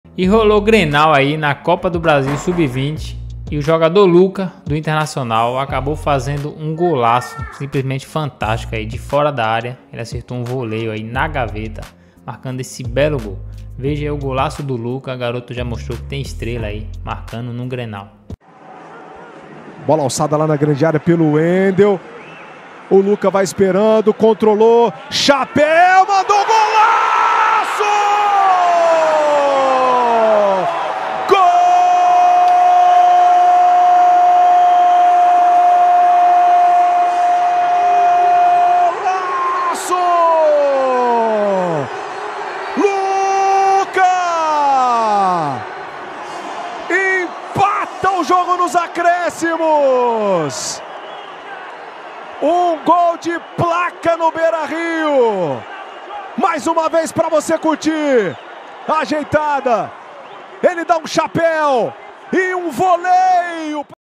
Jogador do Internacional fez um gol antológico contra o Grêmio pela Copa do Brasil Sub20
Lucca marcou nos acréscimos esse golaço de voleio